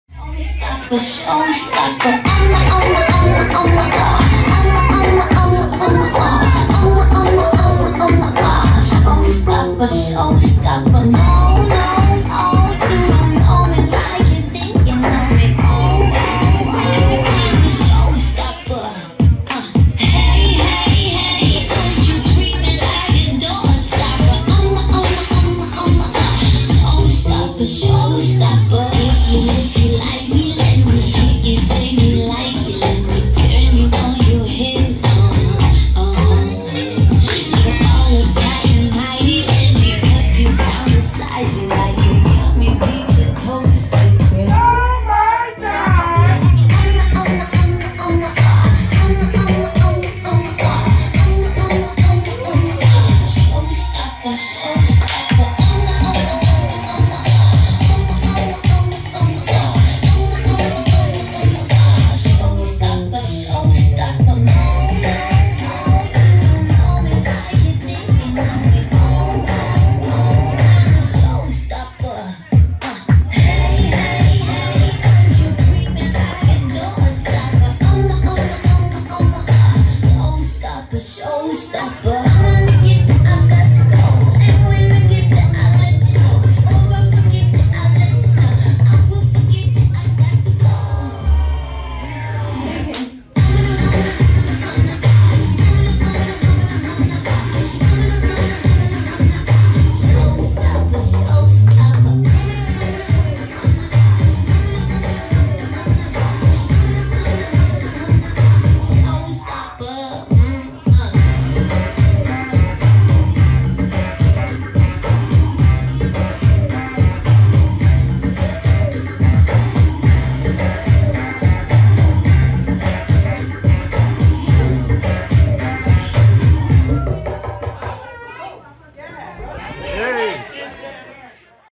Showstopper Line Dance
At @demeriaseguan4117's Make
it DoWhatitDO DwD December Day party & Toy Drive